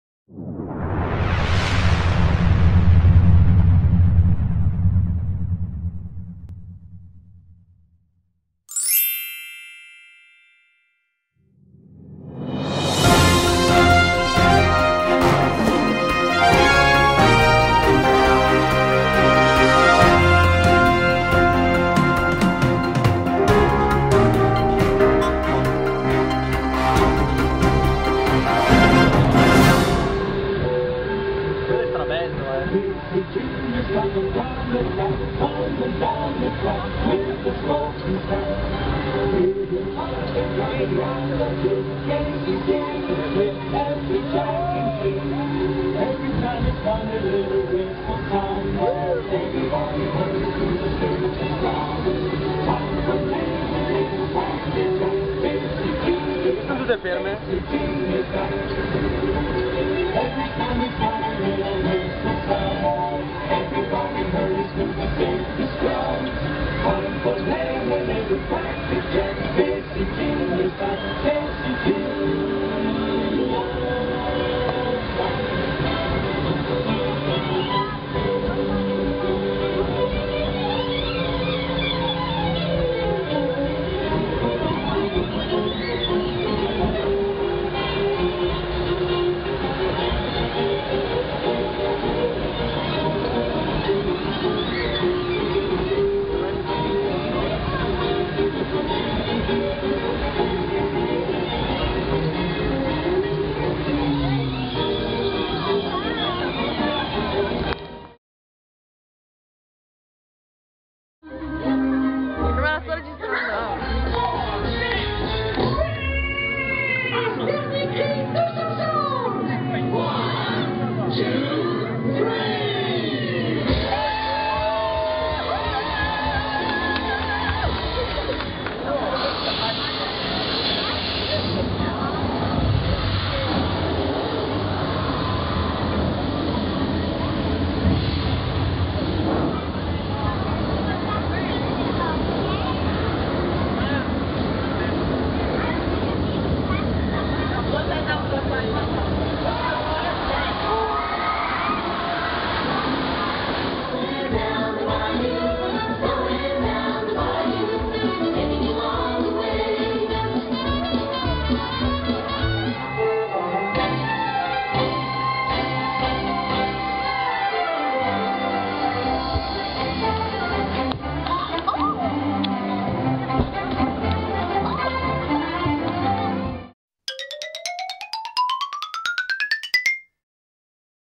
Casey Jr "Disneyland Paris"
Un viaggio sul trenino reso famoso dal film Disney "Dumbo" tra castelli fiabeschi e villaggi incantati. trovare aperta questa attrazione è una rarità visto che il suo settore "misteriosamente" chiude molto presto e spesso è chiuso senza motivo!
Buona visione e. c'è una sorpresa alla fine!